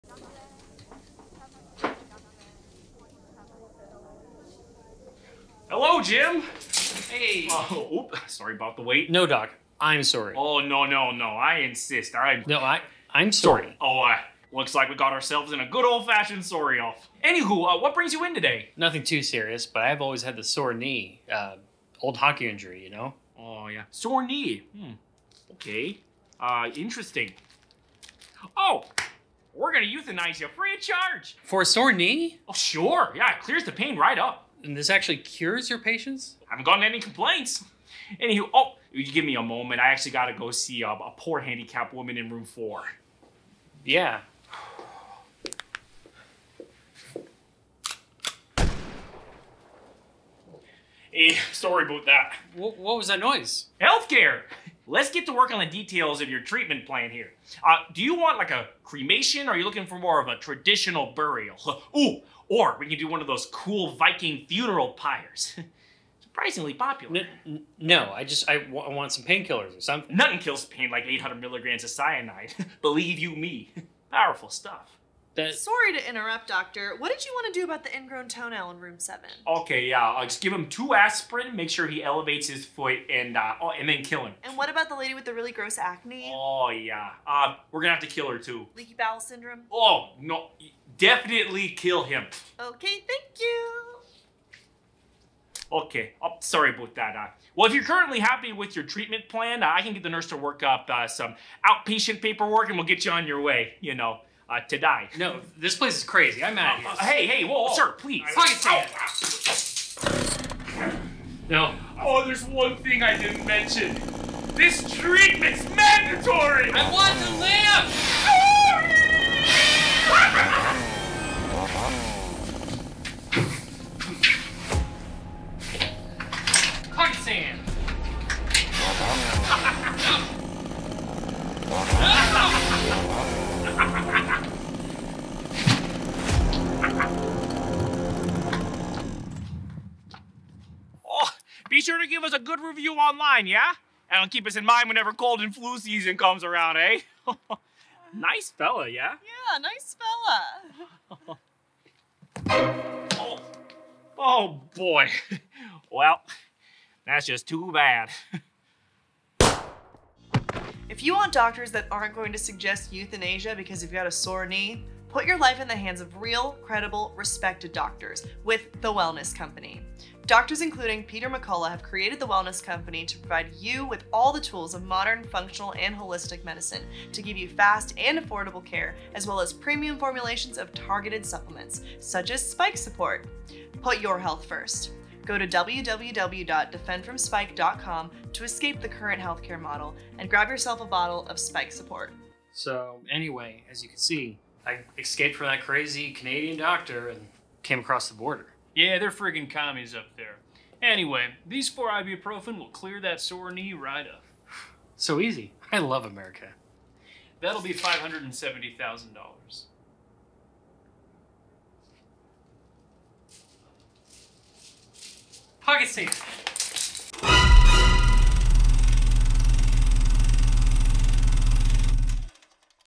Canadian Doctor Prescribes Euthanasia For Sore Knee
Canada's healthcare system has been pushing assisted death programs on its patients. Here's a look inside a routine doctor's appointment in the Great White North.

Put your health first. to escape the current healthcare model, grab yourself a bottle of Spike Support, and put your wellness back in the driver’s seat.